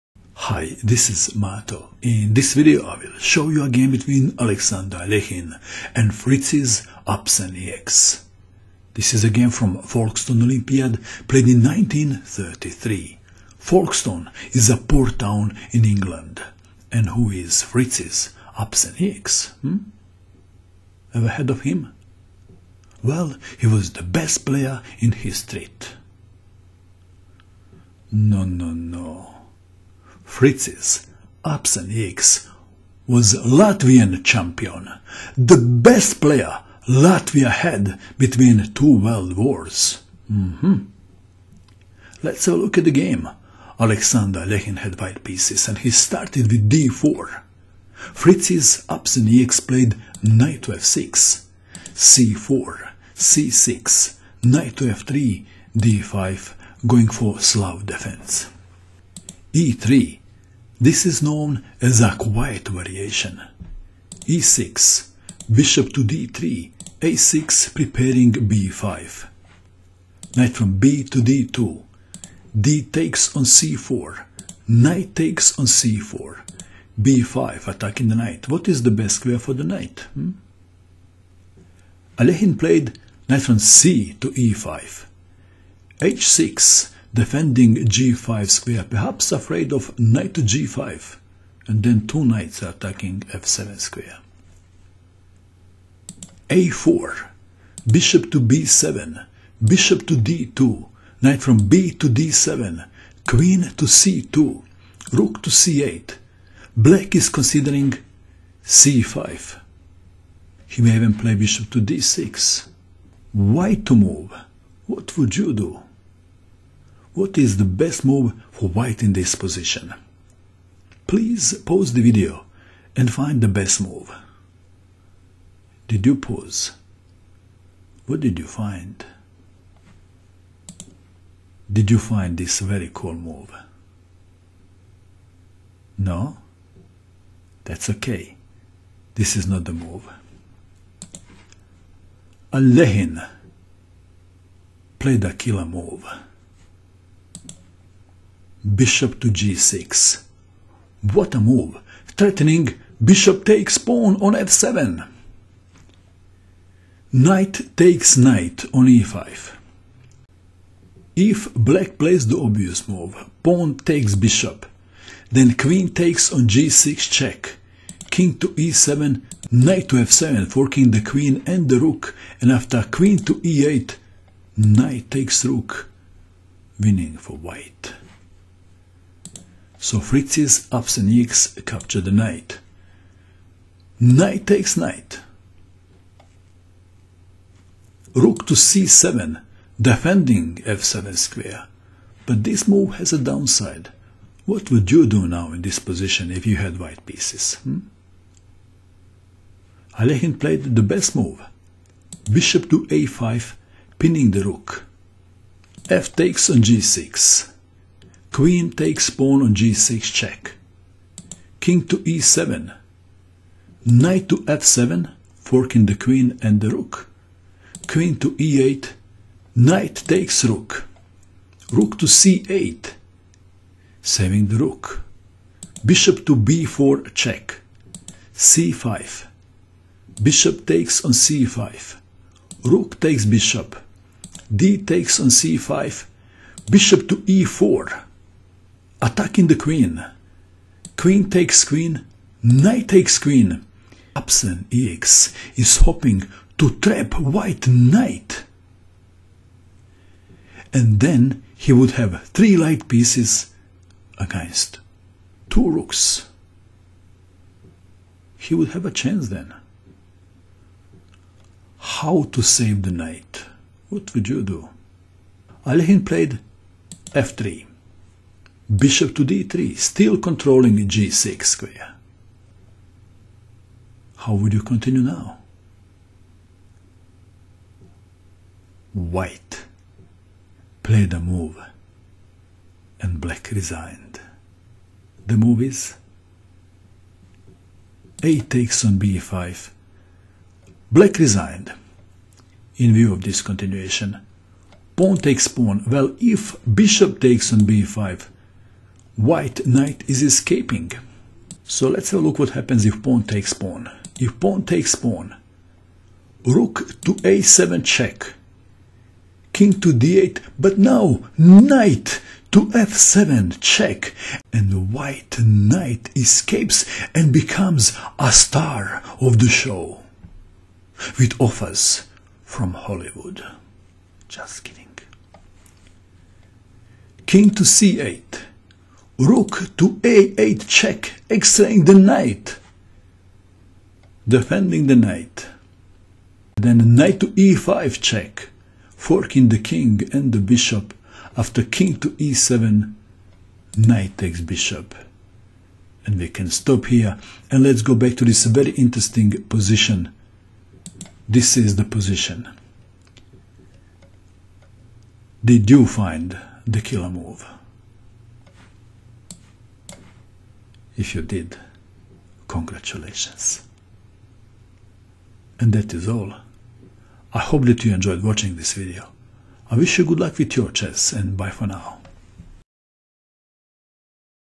Alekhine vs Apsenieks - Folkestone ol 1933
Alexander Alekhine vs Fricis Apsenieks
Folkestone ol (04) 1933
Slav Defense: Quiet Variation (D11)
1. d4 Nf6 2. c4 c6 3. Nf3 d5 4. e3 e6 5. Bd3 a6 6. Nbd2 dc4 7. Nc4 b5 8. Nce5 h6 9. a4 Bb7 10. Bd2 Nbd7 11. Qc2 Rc8 12. Bg6 Ne5 13. Ne5 Rc7 14. Ba5 fg6 15. Qg6 Ke7 16. Nf7 Qe8 17. Nh8 Rc8 18. Bb4 c5 19. Bc5 Rc5 20. dc5 Be4 21. Qe8 Ne8 22. f3 Bd3 23. ab5
 1-0